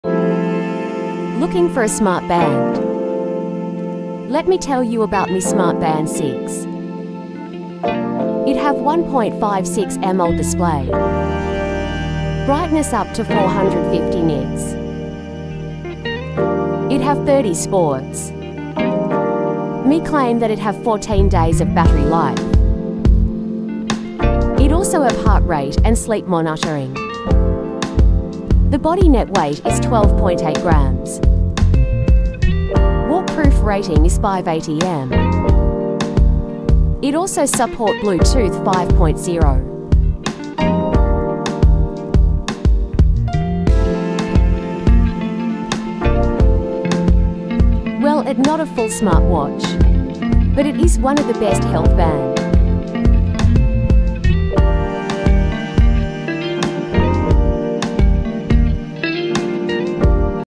Unlocking the Potential: Everything You Need to Know About the Mi Band 6
Discover the Future of Fitness and Connectivity with the Mi Band 6! In this comprehensive video, we delve into all the exciting features and functionalities of Xiaomi's latest smart wearable. From its vibrant AMOLED display to its advanced health tracking capabilities, the Mi Band 6 is designed to empower you on your fitness journey.